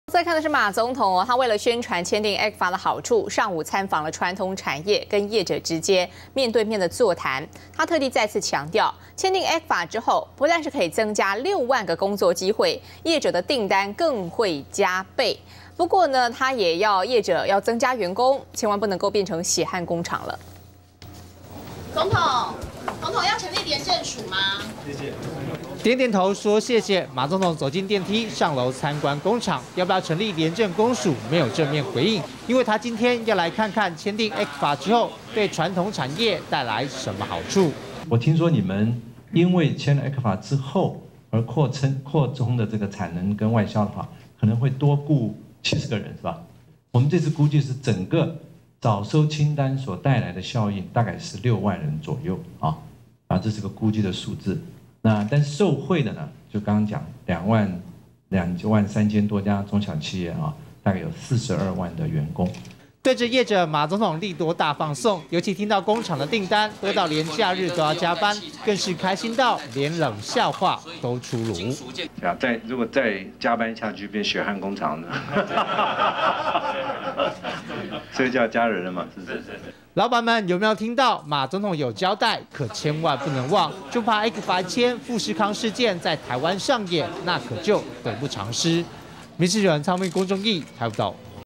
ECFA利多? 馬:增6萬工作－民視新聞
【民視即時新聞】為了宣傳簽訂ECFA的好處，總統馬英九17日上午參訪傳統產業，跟業者面對面座談，他特地強調，簽訂ECFA之後，不但可以增加6萬個工作機會，業者的訂單更會加倍，不過他也笑說，要業者增加員工，千萬不能變成「血汗工廠」!